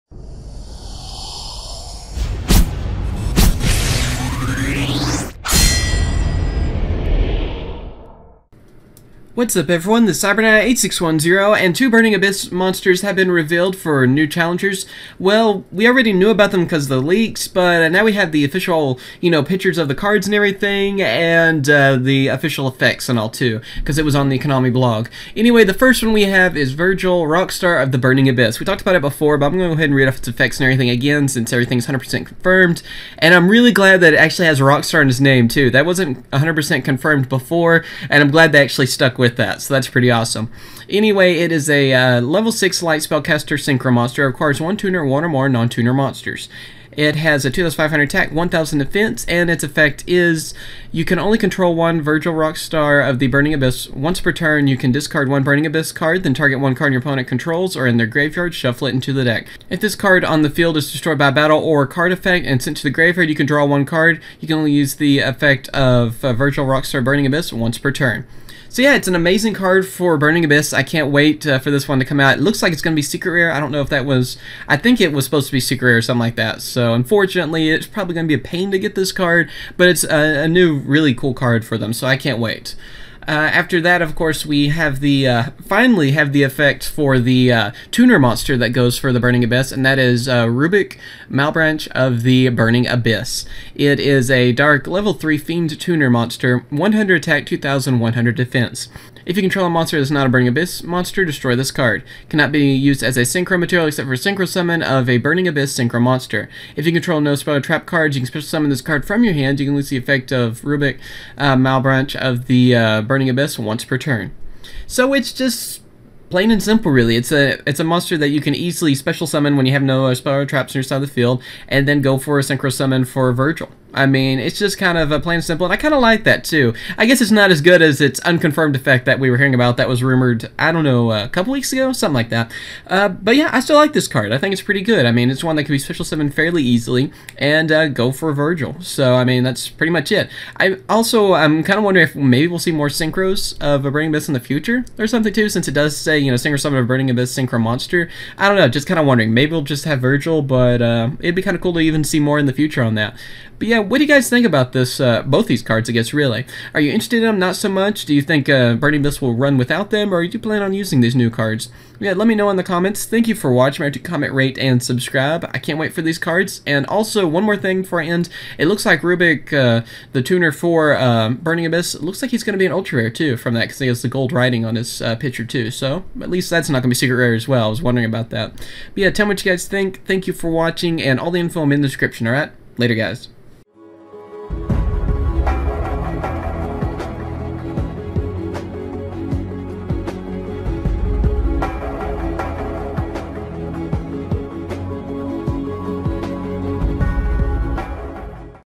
Yugioh NECH Burning Abyss Synchro & Tuner Monster Effects Revealed
We finally have more info on the Burning Abyss cards in The New Challengers, Virgil, Rock Star of the Burning Abyss and Rubic, Malebranche of the Burning Abyss. What do you guys think of these two new cards? I hope you all enjoy the video!
Virgil, Rock Star of the Burning Abyss & Rubic, Malebranche of the Burning Abyss-

Part of The Duelist Alliance